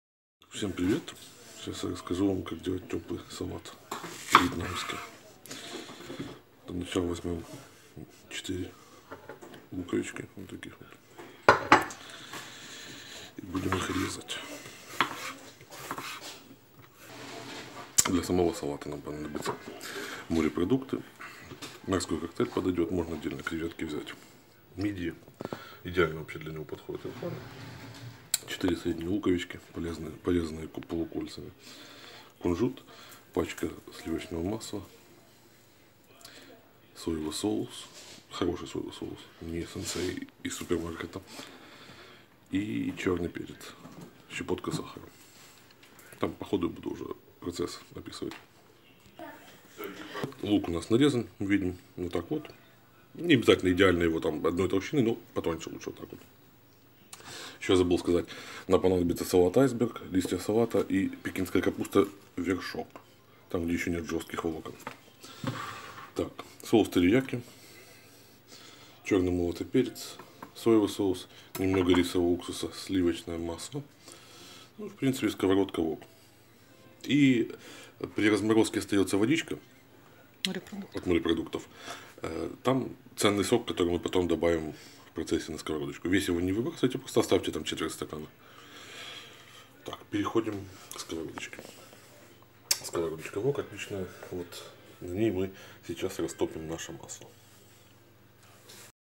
Теплый салат с морепродуктами. Часть первая.
Это первая часть видео-рецепта

Ингридиенты:
Морепродукты 700г
Салат айсберг половина головки
Пекинская капуста 10 листочков
Салат листовой 1 пучок
Соевый соус 30 г
Терияки 20 г
Рисовый уксус 10 г
Черный молотый перец 1 ч.л.

Пошаговый рецепт:
На разогретой сковородке растапливаем масло.
Когда масло растопится,  добавляем морепродукты (самый идеальный салат получался, когда к морскому коктейлю добавляли большие креветки и мясо рапанов).
Через некоторое время добавляем лук.
Когда лук станет мягким, добавляем соусы (в любом порядке) и рисовый уксус.
После этого добавляем кунжут.
Пока  морепродукты и лук тушатся, мы подготовим листья салатов. Их надо нарвать большими кусками на плоскую тарелку.
Через 10 - 15 минут тушения выкладываем на блюдо, перемешиваем и немного поливаем соусом. Приятного аппетита!

Анонсы видео:
Теплый салат с морепродуктами. Продолжение.
Эксперимент: чистый вес морепродуктов без льда.